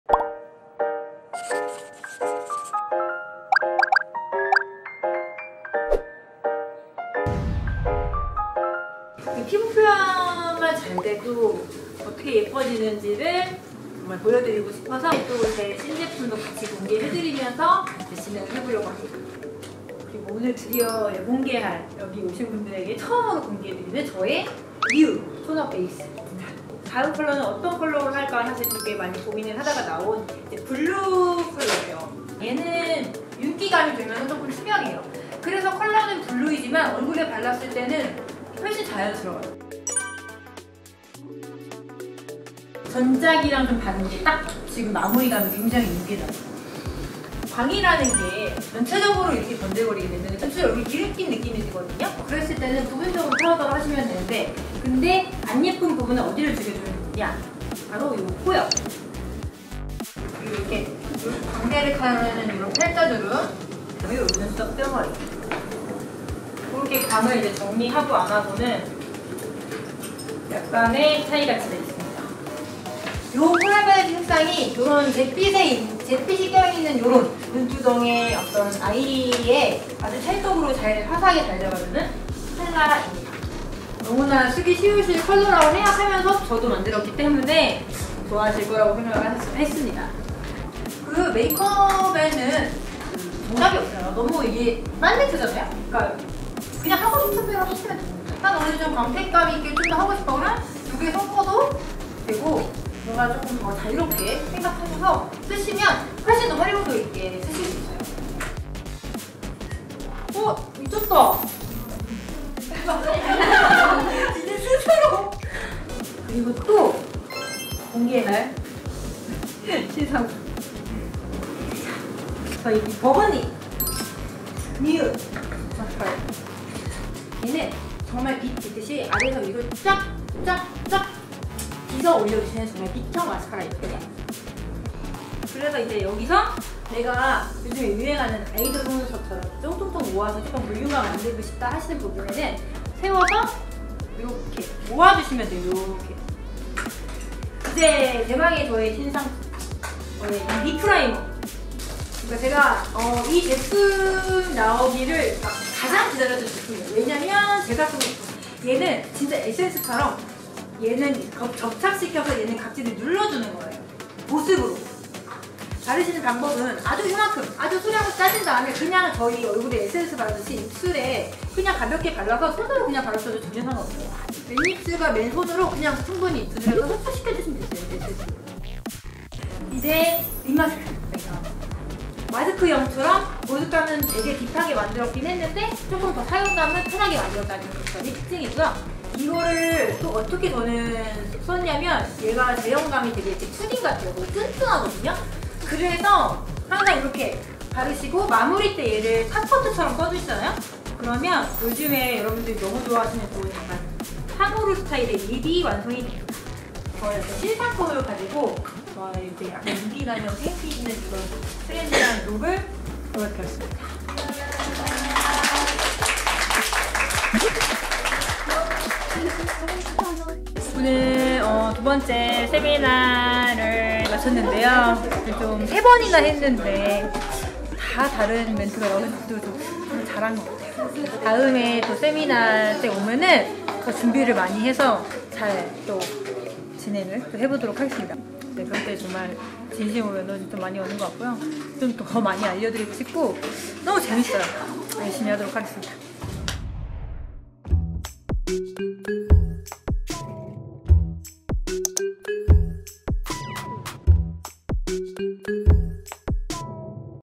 Wonjungyo 신제품이 이렇게나 많이!?🌟ㅣ정요 쌤의 세미나 현장으로 가 보시죠!ㅣWonjungyo behind
여러분!

지난 8월에 진행했던 세미나에서
Wonjungyo 신제품들을 공개했는데요!

톤 업 베이스 블루, 팔레트 블라썸 코랄, 누드 아이래쉬 04 시어 플럼
립 프라이머, 립 마스크 신제품 정말 많죠?ㅎㅎ

세미나에서 늘 에너지를 얻고, 행복을 느끼며
즐거운 경험을 얻어가는 것 같습니다.
앞으로도 많은 관심 부탁드리겠습니다!

💫Wonjungyo 1st Makeup Book💫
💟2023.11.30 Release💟

Amazon Japan